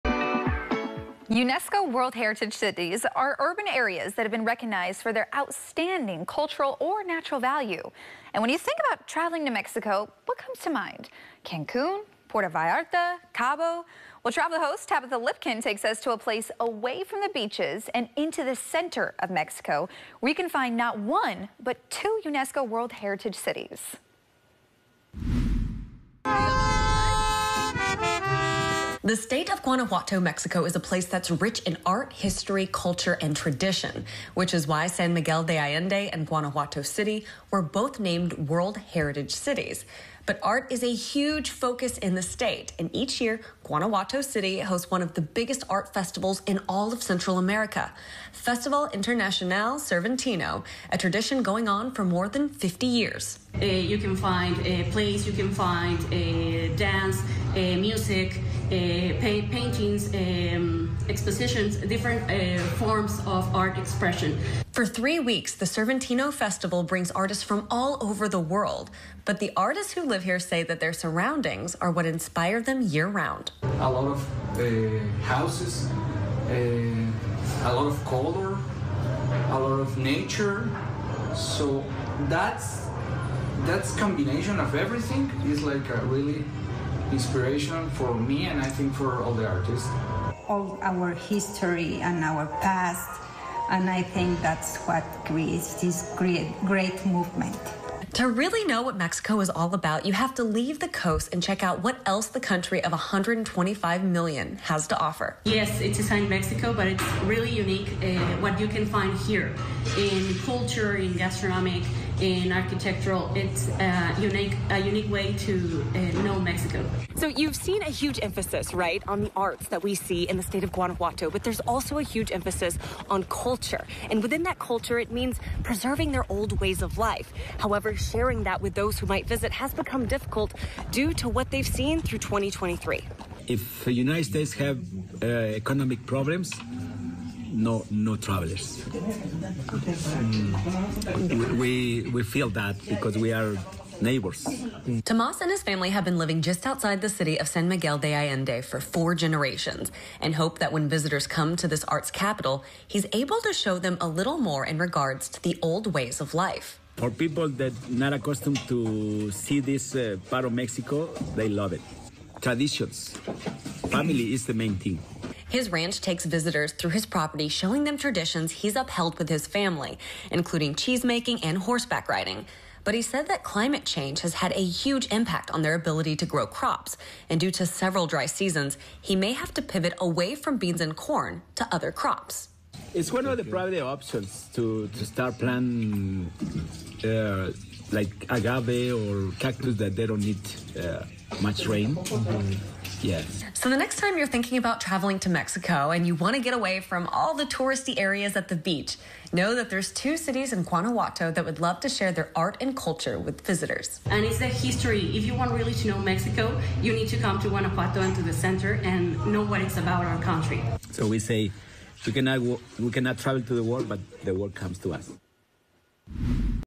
Exploring UNESCO World Heritage Cities in Mexico
Travel Host Tabitha Lipkin takes us into the center of Mexico to explore where you can find not one, but two UNESCO world heritage cities.